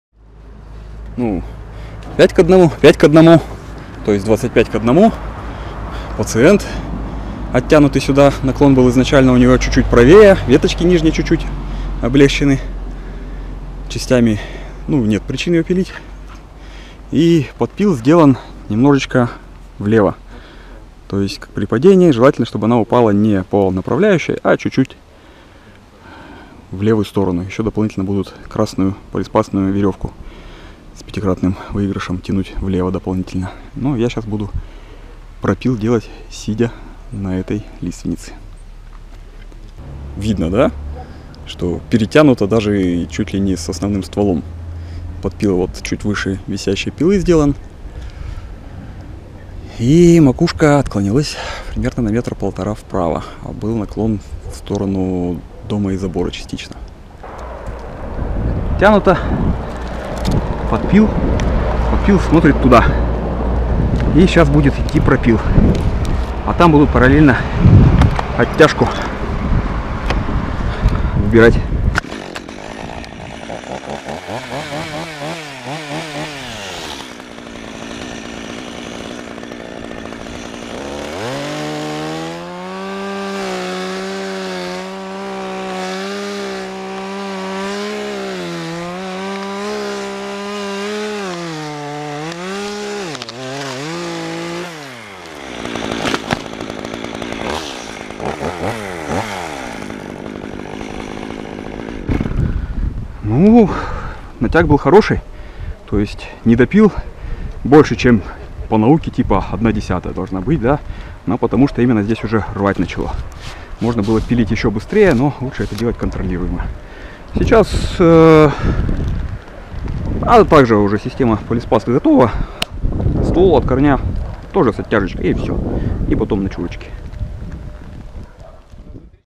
Валим пополам
Снос дерева на месте будущего строительства. Лиственница была ранее варварски обрезана энергетиками. Кедр должен остаться цел!
По ряду причин дерево не разбирается по частям, а валится в два приёма с отклоняющей полиспастной оттяжкой.

Пригород Иркутска.